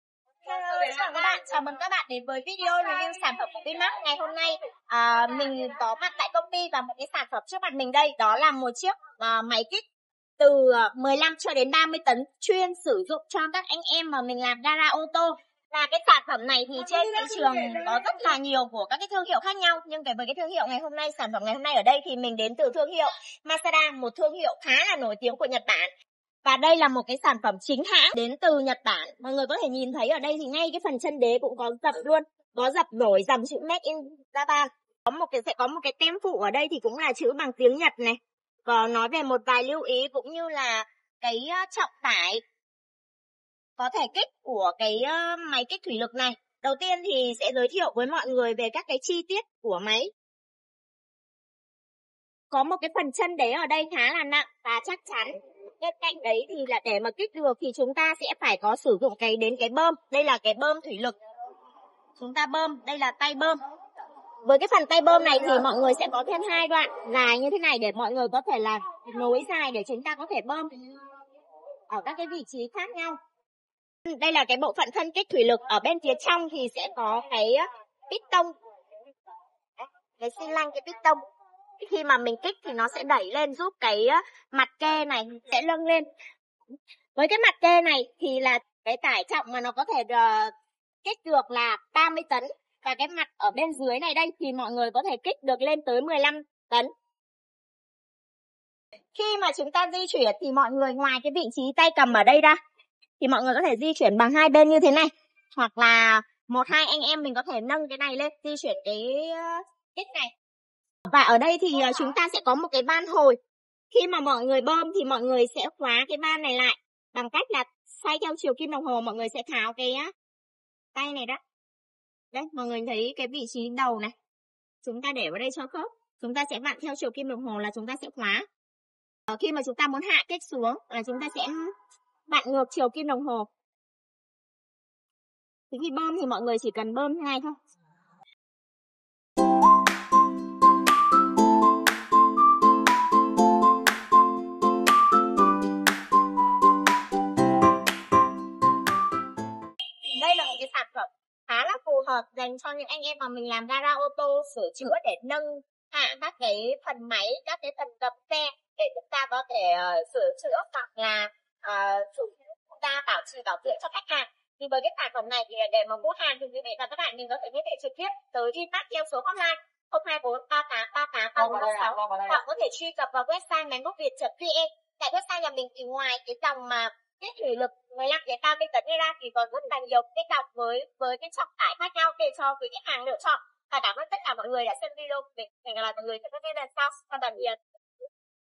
Review - Kích thuỷ lực móc 15 tấn Masada MHC-15T vinmax / LH 0974813007
Tải trọng nâng của đầu : 30 tấn
Tải trọng nâng của chân : 15 tấn
Nâng thấp nhất (chân ) : 32mm
Nâng thấp nhất (đầu) : 345mm
Hành trình nâng: 140mm
Nâng cao nhất (chân) : 172mm
Nâng cao nhất (đầu) : 485mm
Kích thước đế : 258mm x 321mm
Kích thước chân: 110mm x 60mm
Trọng lượng : 53 kg